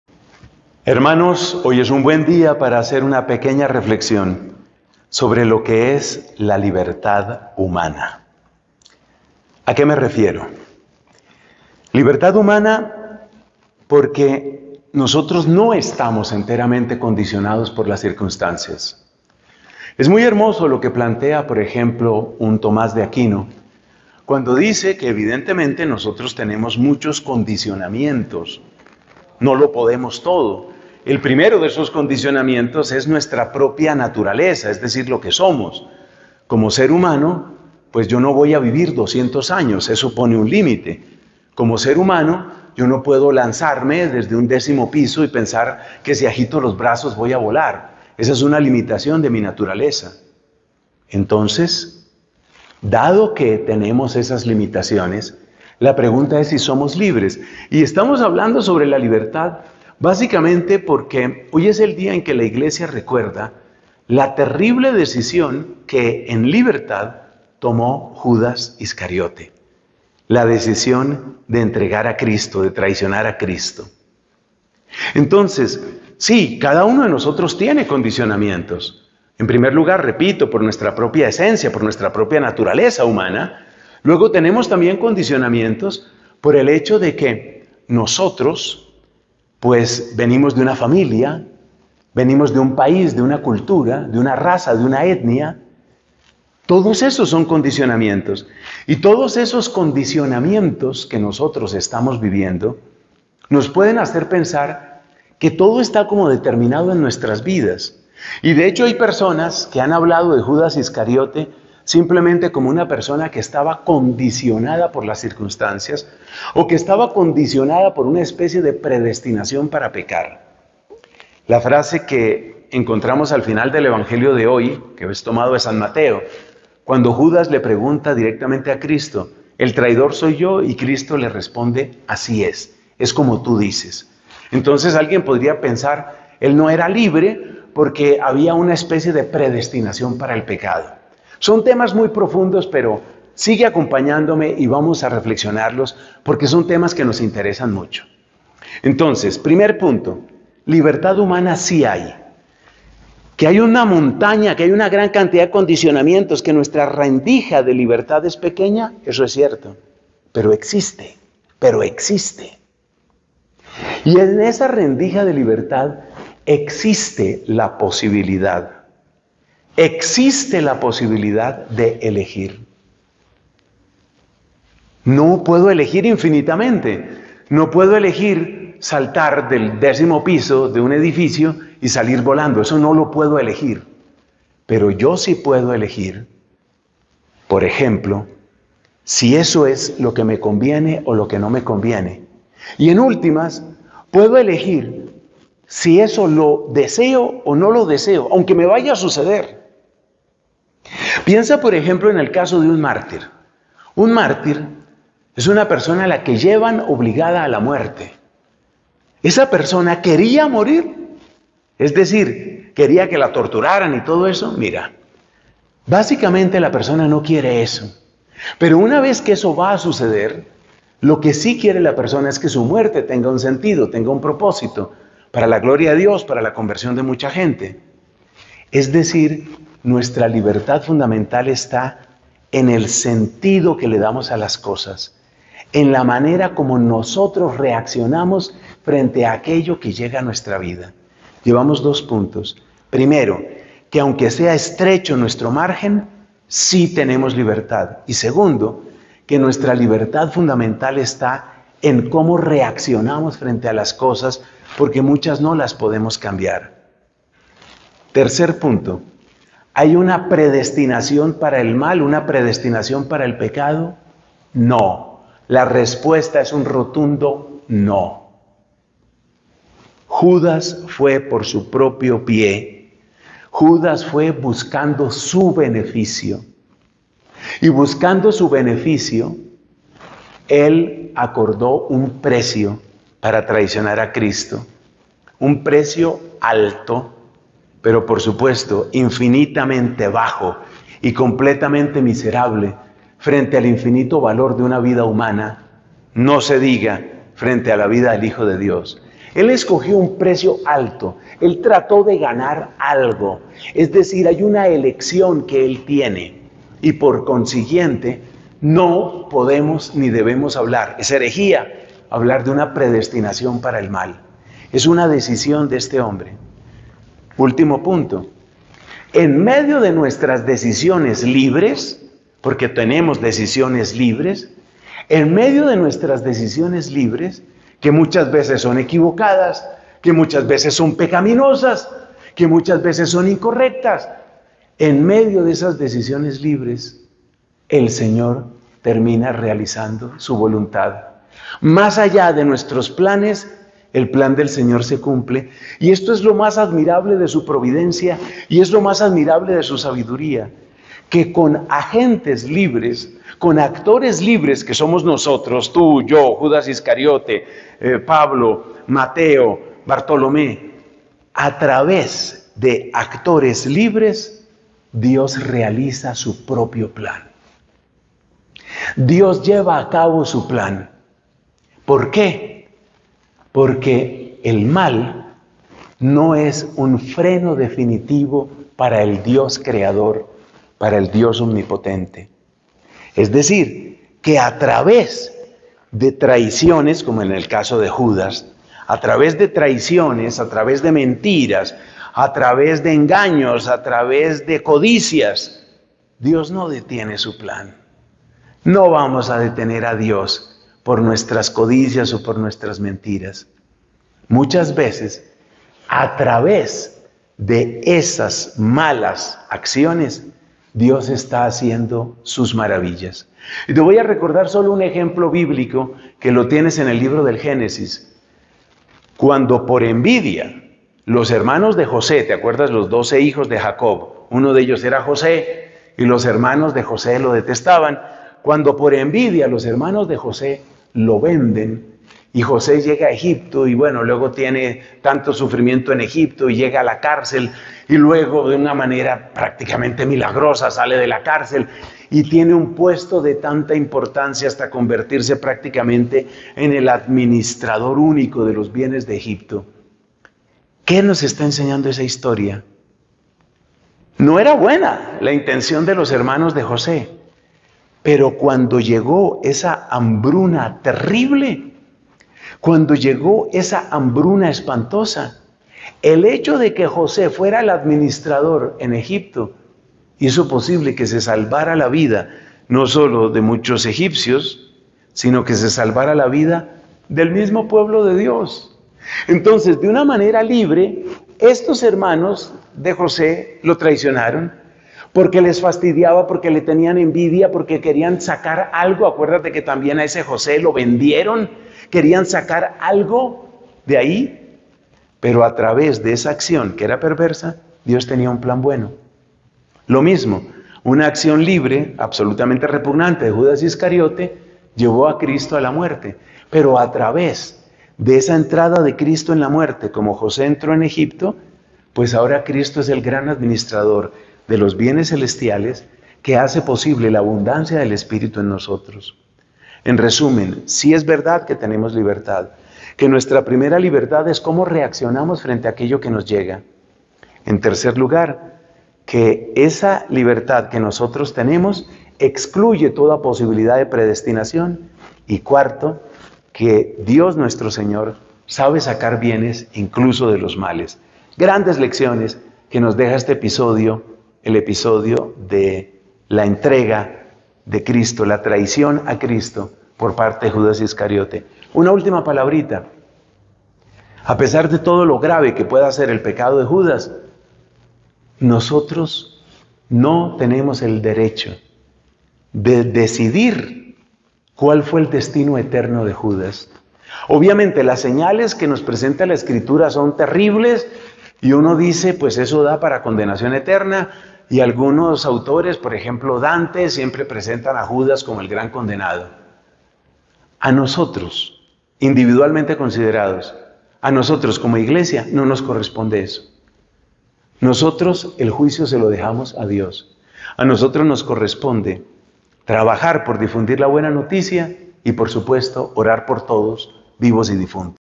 LA GRACIA 2023/04/05 ¿Qué es la libertad humana?
Tenemos libertad lo que implica nuestra reacción frente a lo que nos llega, también excluye toda posibilidad de predestinación y a pesar de nuestras decisiones libres Dios al final realiza su Voluntad.